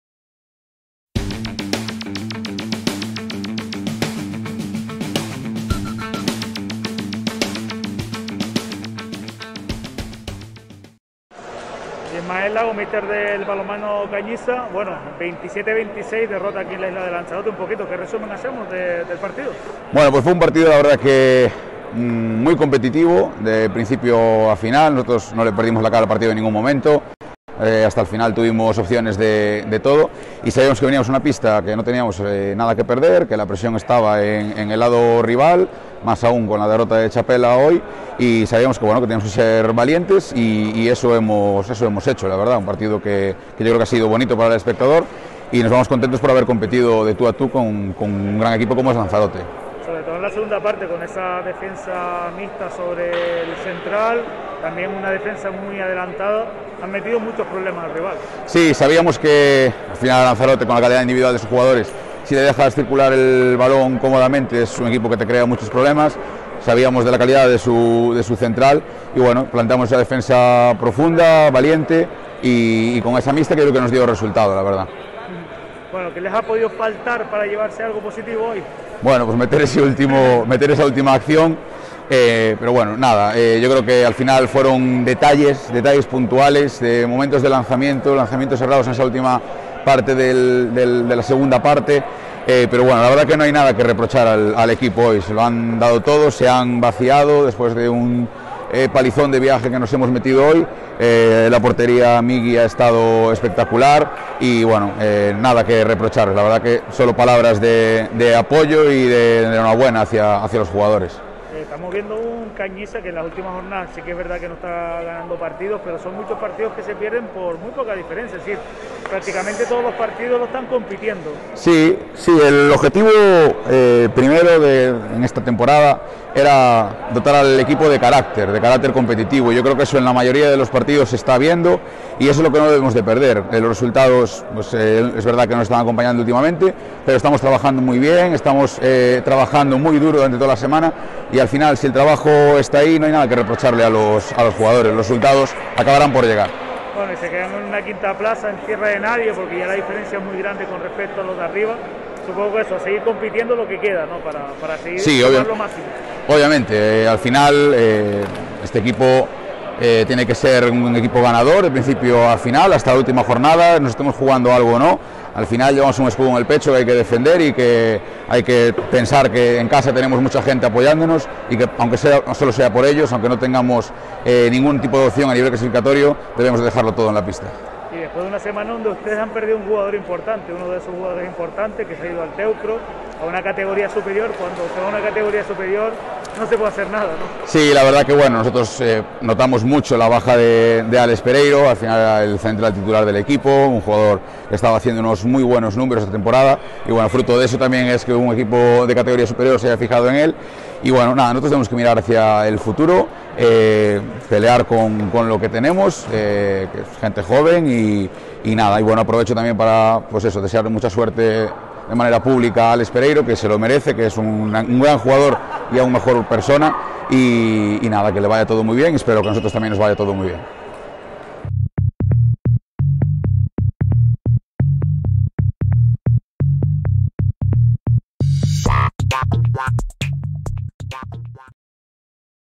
Ismael Lago. San José Obrero vs Balonmano Cañiza
Entrevista al entrenador del Balonmano Saeplast Cañiza, Ismael Lago, a la conclusión del encuentro disputado ante el Gourmet Ampate Lanzarote, correspondiente a la 22º jornada de la Primera Nacional Masculina.